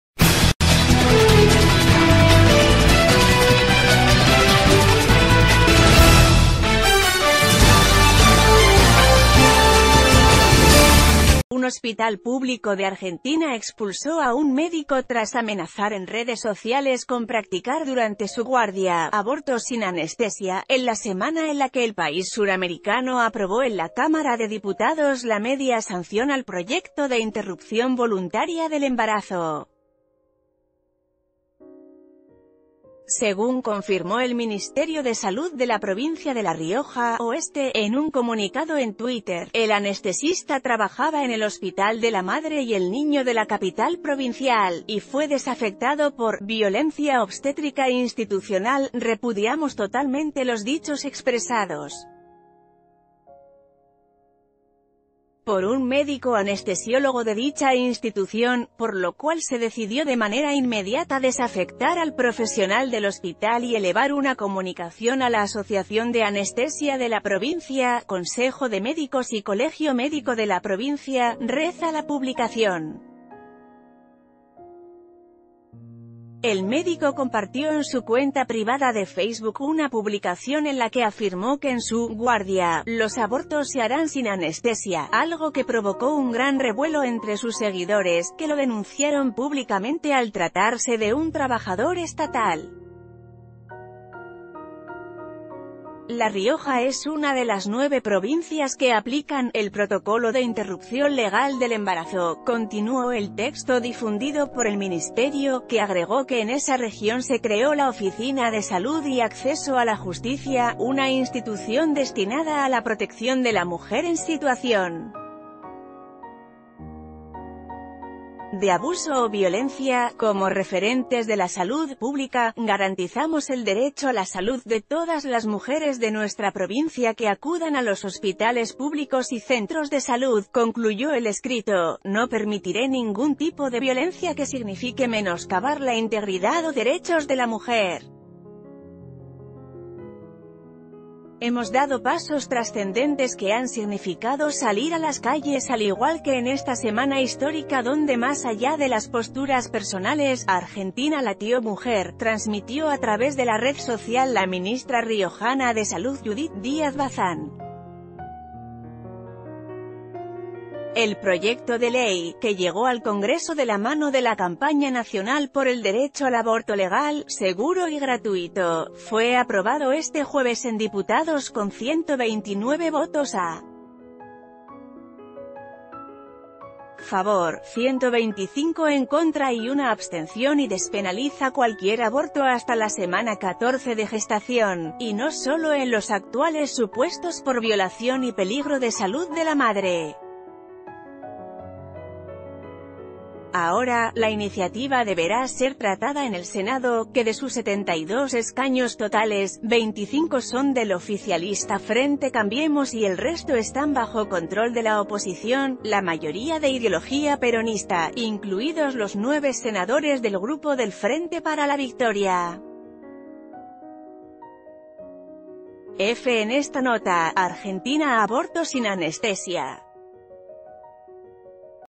Médico amenazó con practicar "abortos sin anestesia"
Un hospital público de Argentina expulsó a un médico tras amenazar en redes sociales con practicar durante su guardia "abortos sin anestesia", en la semana en la que el país suramericano aprobó en la Cámara de Diputados la media sanción al proyecto de Interrupción Voluntaria del Embarazo. Según conf...